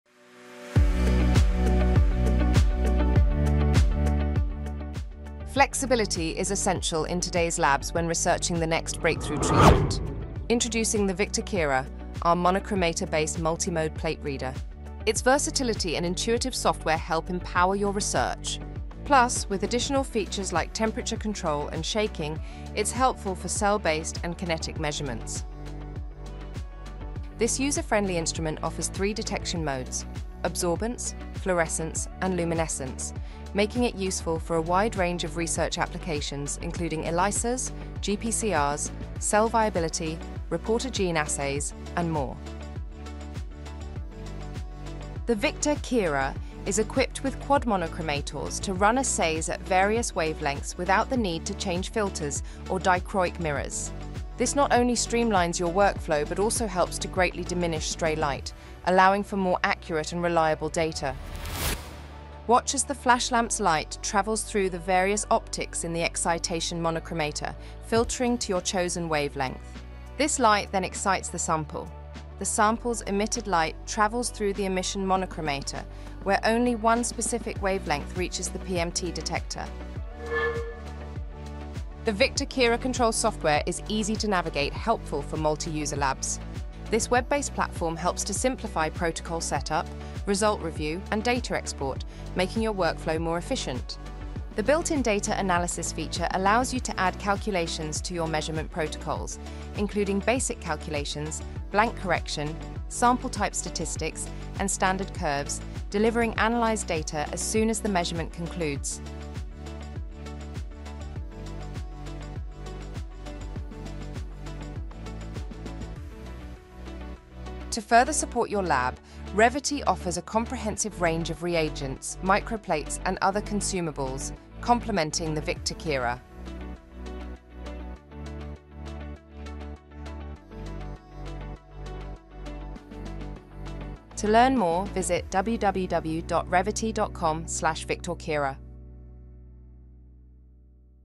VICTOR Kira Multimode Plate Reader
The VICTOR Kira™ multimode plate reader delivers versatile research capabilities in a compact, lab-friendly design. Featuring triple detection (absorbance, fluorescence, luminescence), advanced monochromator technology, and intuitive software, it streamlines workflows while maximizing lab efficiency. Backed by Revvity's comprehensive assay portfolio.